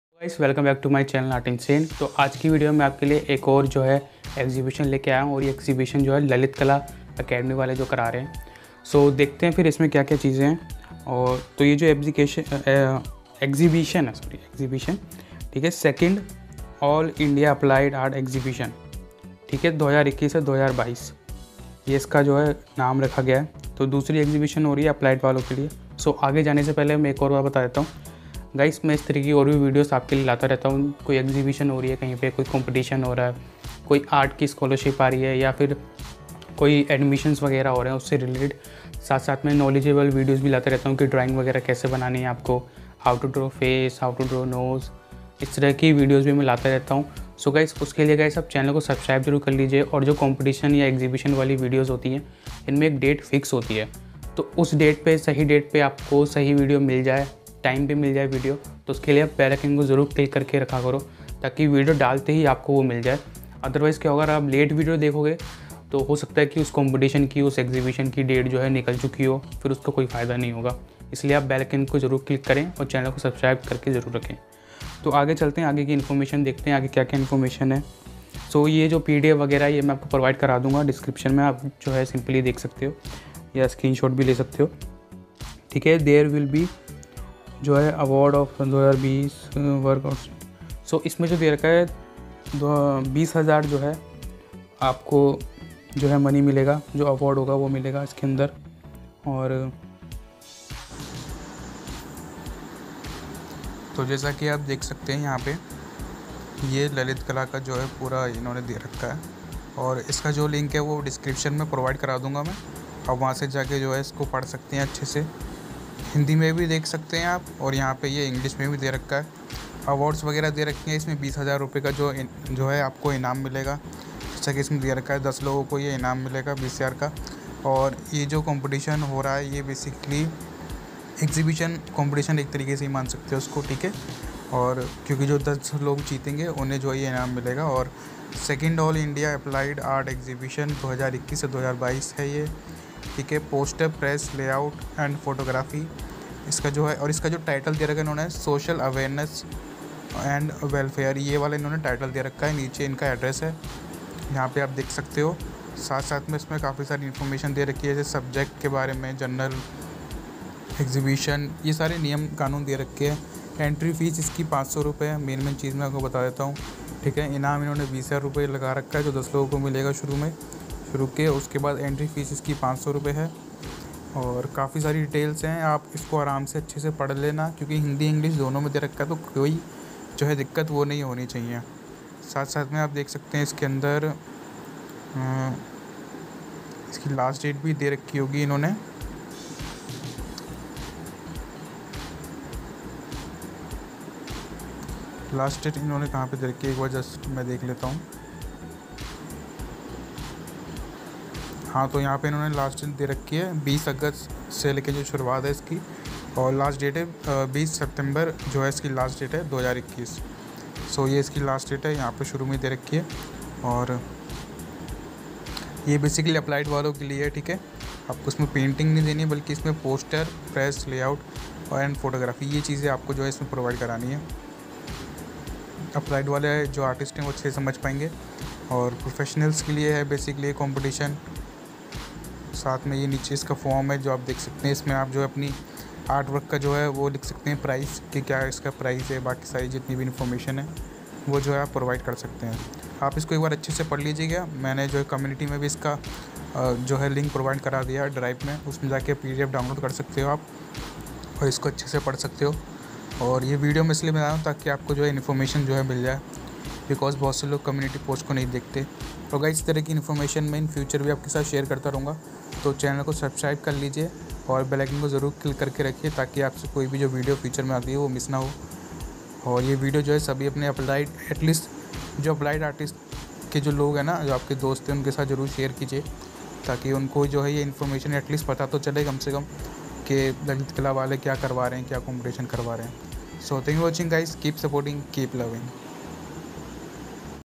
Lalit kala ART EXHIBITON 2021-2022 ( Art Competition 2021)
New Art Exhibition /Art Competition By Lalit Kala  See Full Pdf On Drive and Read All rules of participation  Best' of luck to all👍